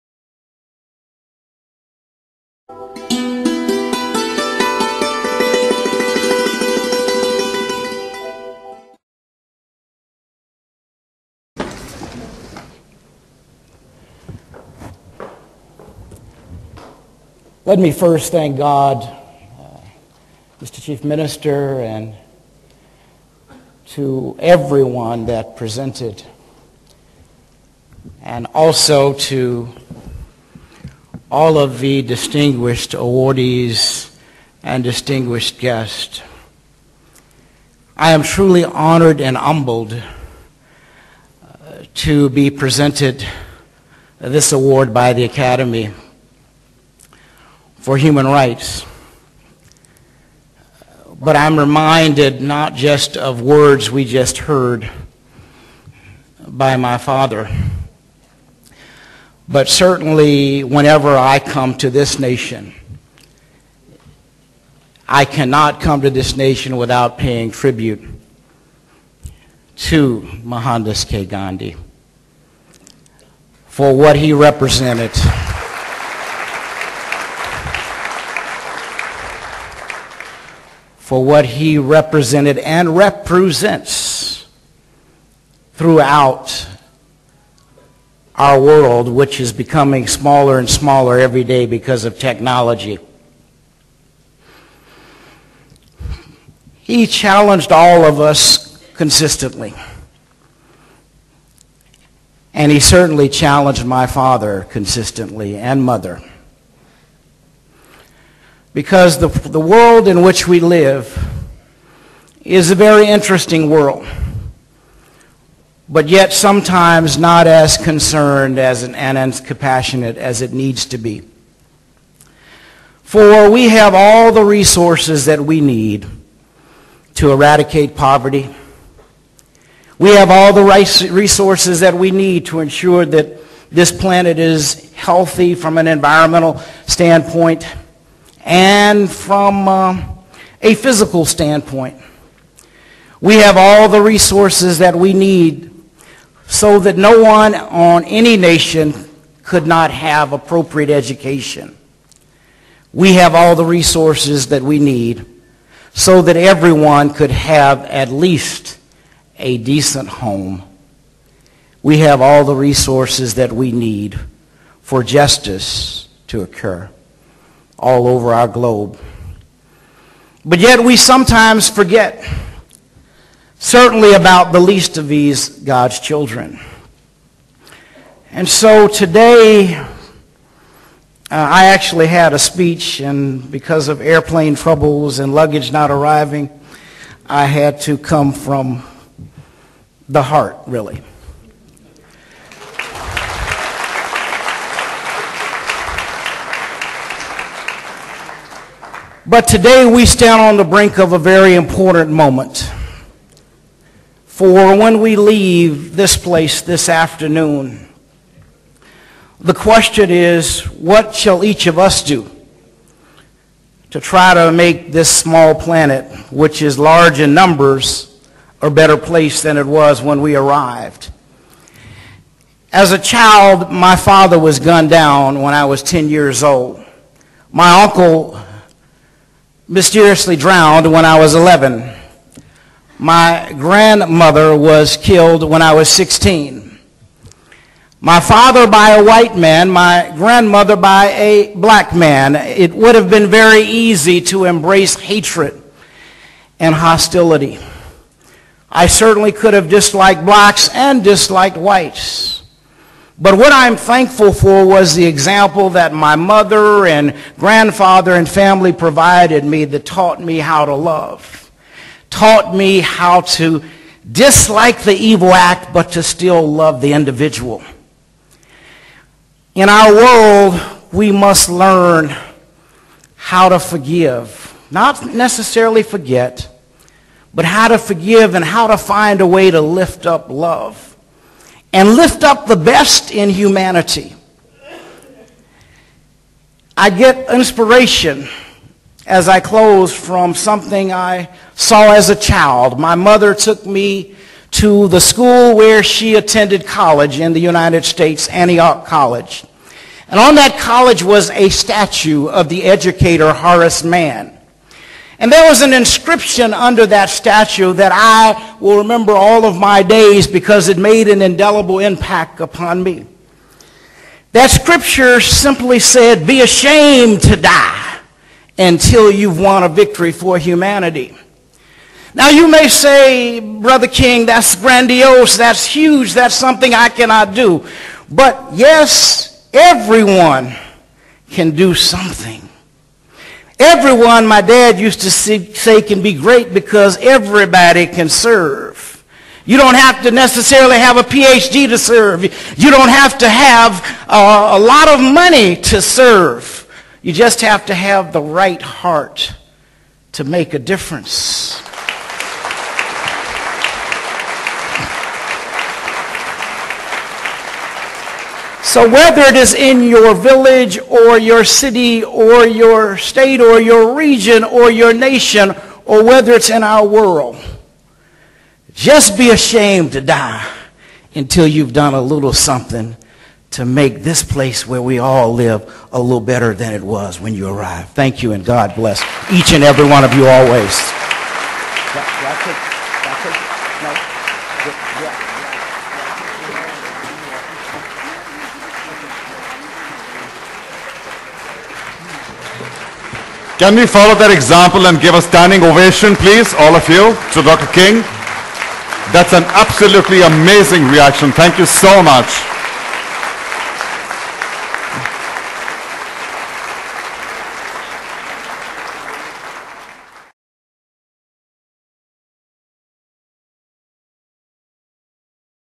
Mr. Martin Luther King III Speech.wmv
Mr. Martin Luther King III speaking at the 26th. Anniversary Global Awards Function of Priyadarshni Academy at Regal Room, Trident Hotel on 19th. September 2010